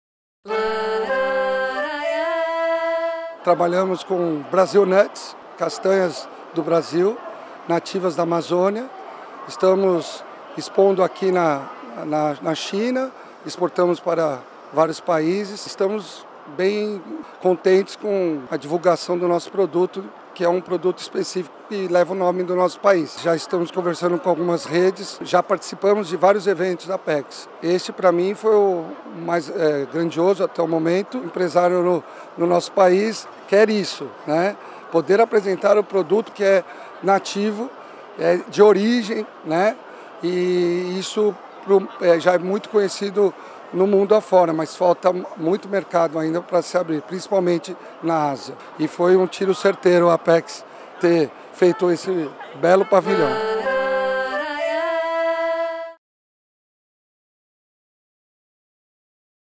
Ricardo Rosa, da Amazon Group, fala sobre a participação na CIIE
Ricardo Rosa, CEO da Amazon Group, fala sobre a promoção dos produtos brasileiros no mercado chinês durante a CIIE, maior feira de importação do mundo, realizada entre 5 e 10 de novembro em Xangai.

O Grupo de Trabalho da CIIE 2018 foi composto por: MRE, MAPA, MDIC, Mtur, Embratur, CNI, CNA, FIESP e Banco do Brasil.